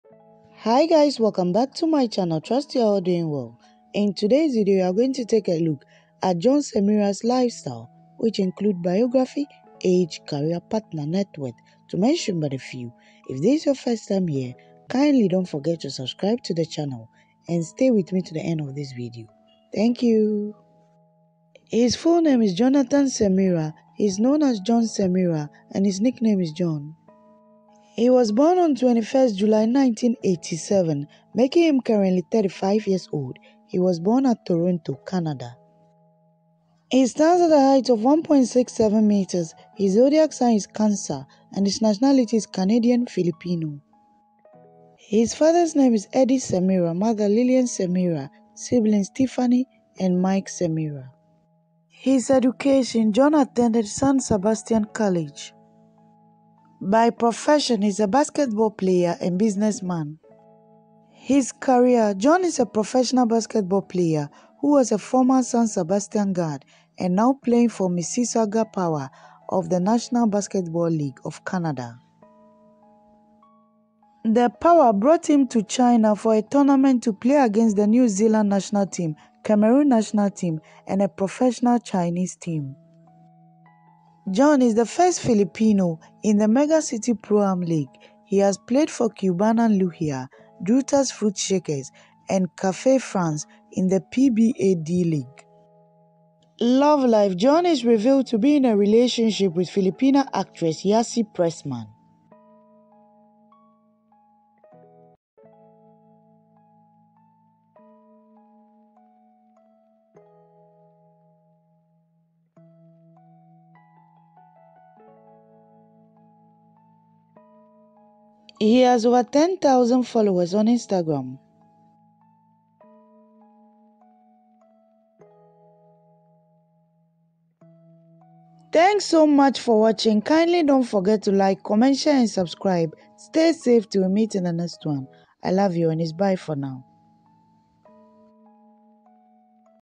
Jon Semira (Yassi Pressman's New Boyfriend) Lifestyle 2022 || Biography, Career, Net worth, Cars
This video gives the details of the lifestyle(Biography, Real Name, Age, Partner, Career and Networth) of Popular Filipino Basketball Player Jon Semira Boyfriend of Actress
Yassi Pressman